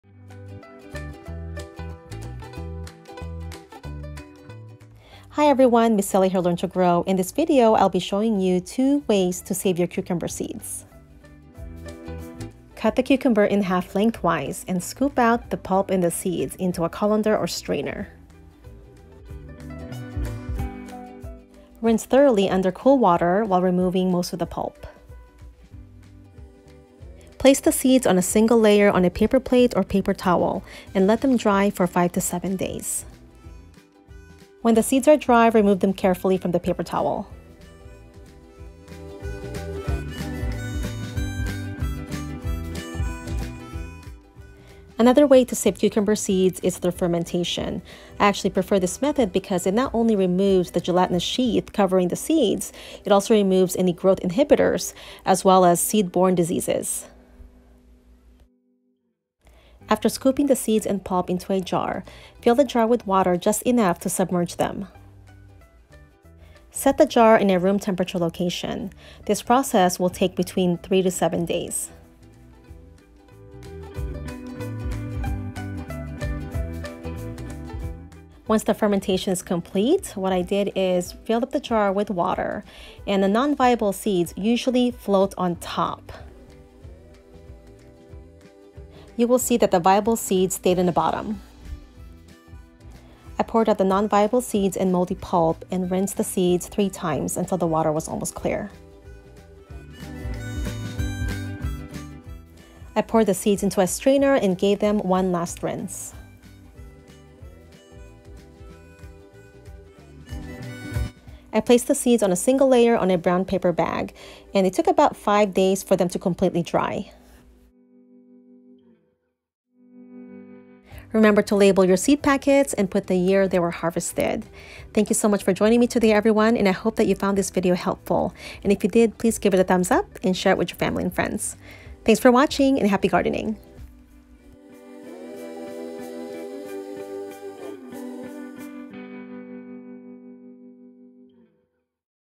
How to Save Cucumber Seeds
How to Save Cucumber Seeds. Saving cucumber seeds for planting or sharing.

Smartpots Fabric Conttainers
Greenhouse, Cold Frame, Tunnel, Row Cover
Seeds for Microgreens: (BULK)
Soil, Compost or Potting Mix
Organic Fertilizer and Cuttings
Garden Tools
Kids' Garden Tools
Light Fixtures Farming